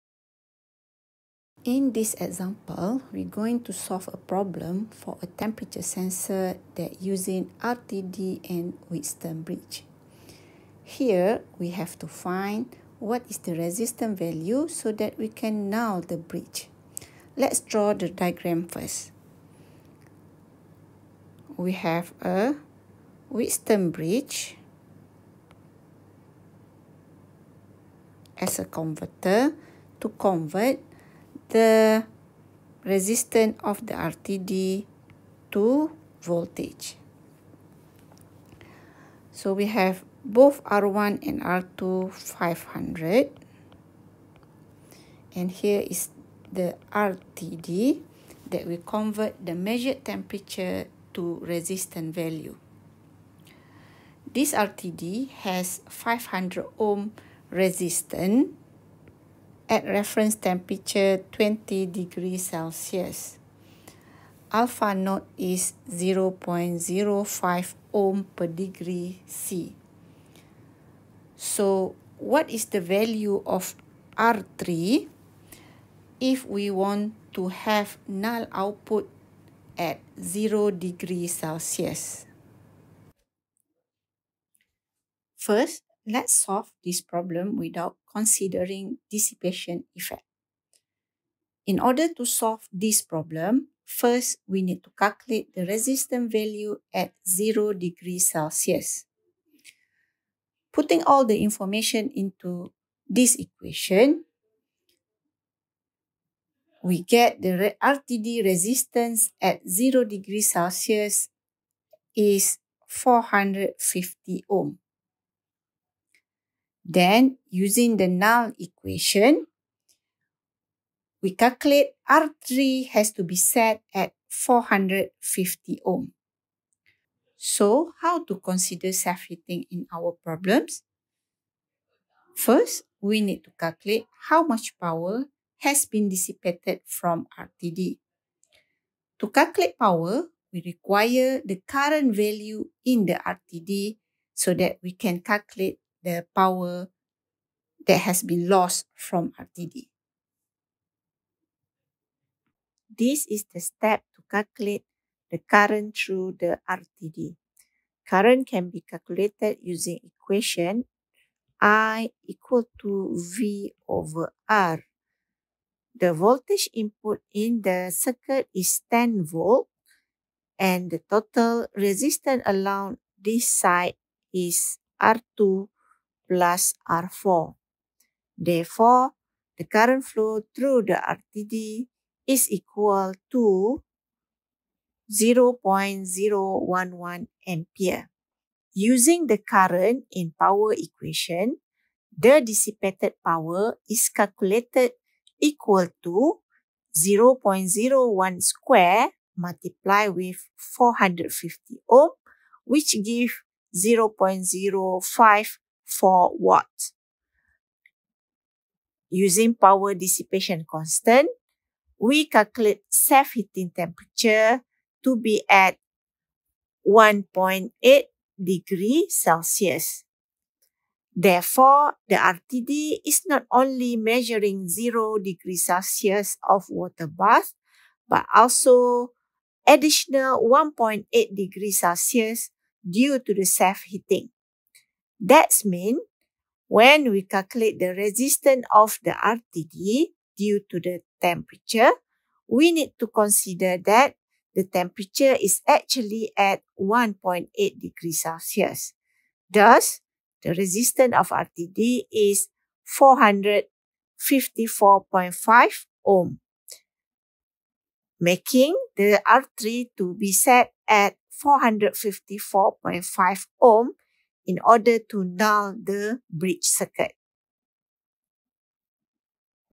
Example 1 (RTD)
This example discuss on the effect of self heating RTD